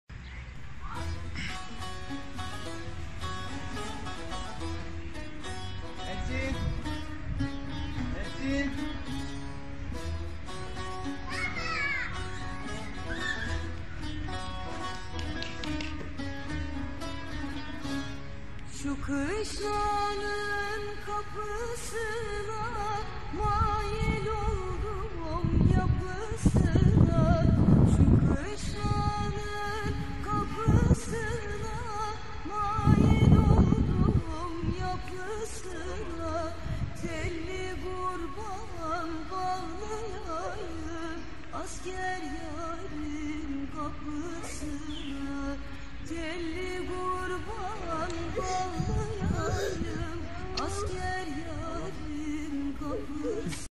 Afrinden dönen askerin kızıyla kavuşma anı rabbim tüm askerlerimizi korusun
🌟Afrin'den dönen askerimizle kızının buluşma anı...

Allah her askerimizi, polisimizi evine, ailesine sağsalim kavuştursun. Amin👐💟🙋‍♀️